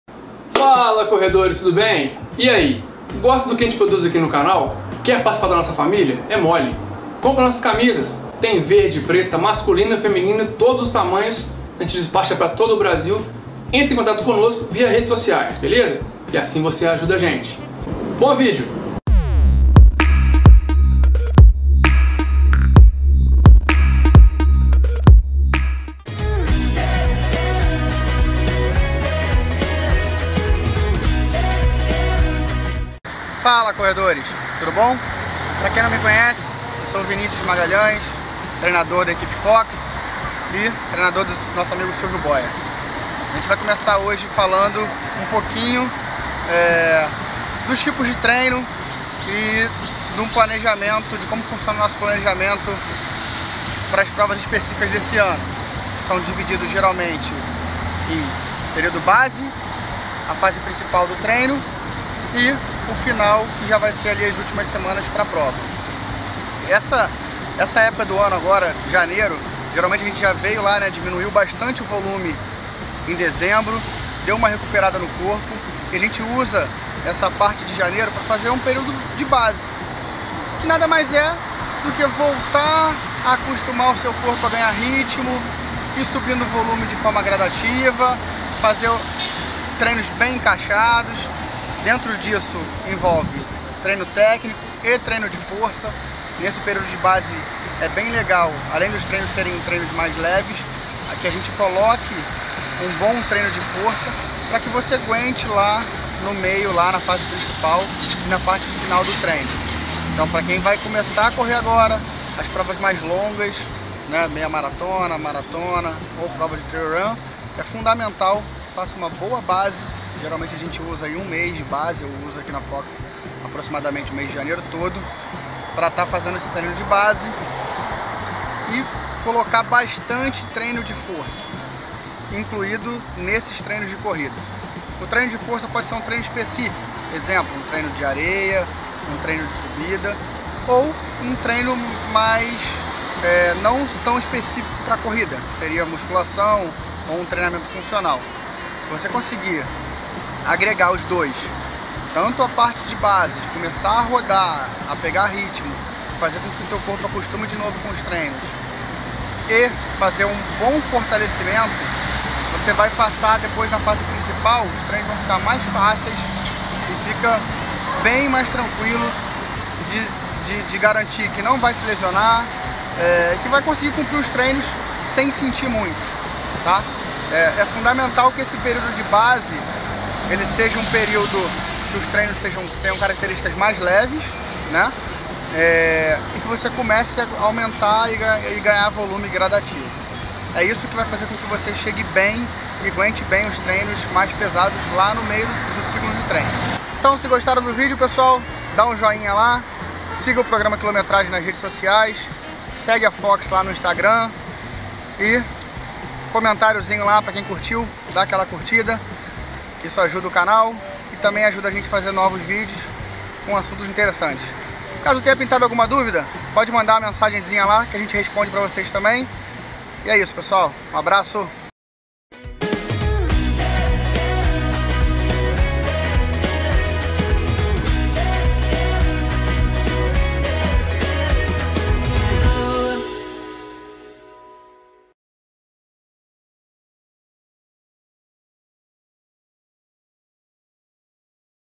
Como fazer TREINOS DE BASE
Faaaaaala corredores, tudo bem? Estamos começando novamente a temporada e com isso o período de base. Você sabe o que é? Assista então o vídeo com nosso treinador Vini Magalhães e saiba mais.